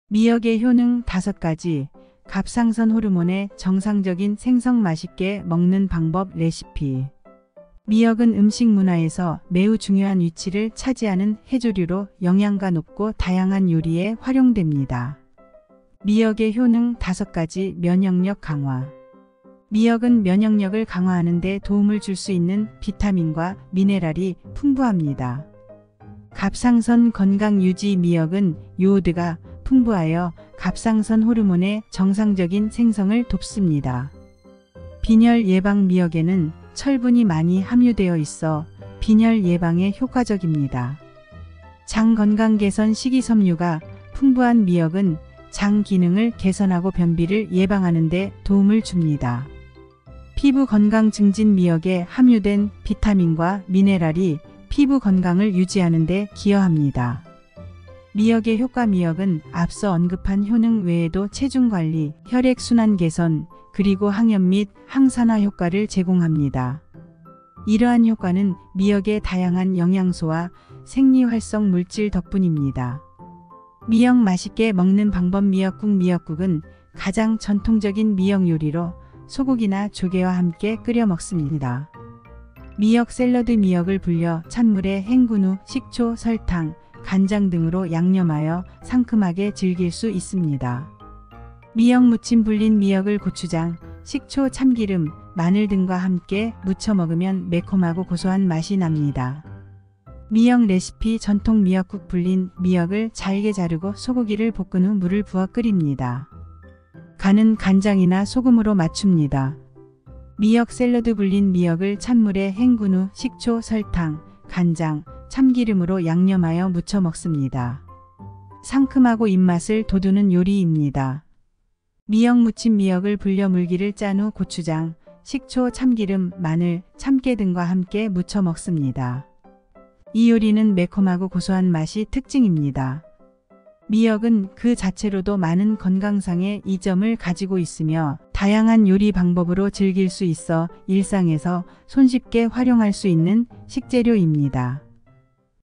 미역의 효능 5가지, 갑상선 호르몬의 정상적인 생성 I 맛있게 먹는 방법 I 레시피
미역은 음식 문화에서 매우 중요한 위치를 차지하는 해조류로, 영양가 높고 다양한 요리에 활용됩니다.
미역의 효능 5가지: 면역력 강화: 미역은 면역력을 강화하는 데 도움을 줄 수 있는 비타민과 미네랄이 풍부합니다.
갑상선 건강 유지: 미역은 요오드가 풍부하여 갑상선 호르몬의 정상적인 생성을 돕습니다.
빈혈 예방: 미역에는 철분이 많이 함유되어 있어 빈혈 예방에 효과적입니다.
장 건강 개선: 식이섬유가 풍부한 미역은 장 기능을 개선하고 변비를 예방하는 데 도움을 줍니다.
피부 건강 증진: 미역에 함유된 비타민과 미네랄이 피부 건강을 유지하는 데 기여합니다.
미역의 효과: 미역은 앞서 언급한 효능 외에도 체중 관리, 혈액 순환 개선, 그리고 항염 및 항산화 효과를 제공합니다. 이러한 효과는 미역의 다양한 영양소와 생리활성 물질 덕분입니다.
미역 맛있게 먹는 방법: 미역국: 미역국은 가장 전통적인 미역 요리로, 소고기나 조개와 함께 끓여 먹습니다.
미역 샐러드: 미역을 불려 찬물에 헹군 후, 식초, 설탕, 간장 등으로 양념하여 상큼하게 즐길 수 있습니다.
미역 무침: 불린 미역을 고추장, 식초, 참기름, 마늘 등과 함께 무쳐 먹으면 매콤하고 고소한 맛이 납니다.
미역 레시피: 전통 미역국: 불린 미역을 잘게 자르고, 소고기를 볶은 후 물을 부어 끓입니다. 간은 간장이나 소금으로 맞춥니다.
미역 샐러드: 불린 미역을 찬물에 헹군 후, 식초, 설탕, 간장, 참기름으로 양념하여 무쳐 먹습니다. 상큼하고 입맛을 돋우는 요리입니다.
미역 무침: 미역을 불려 물기를 짠 후, 고추장, 식초, 참기름, 마늘, 참깨 등과 함께 무쳐 먹습니다. 이 요리는 매콤하고 고소한 맛이 특징입니다.
미역은 그 자체로도 많은 건강상의 이점을 가지고 있으며, 다양한 요리 방법으로 즐길 수 있어 일상에서 손쉽게 활용할 수 있는 식재료입니다.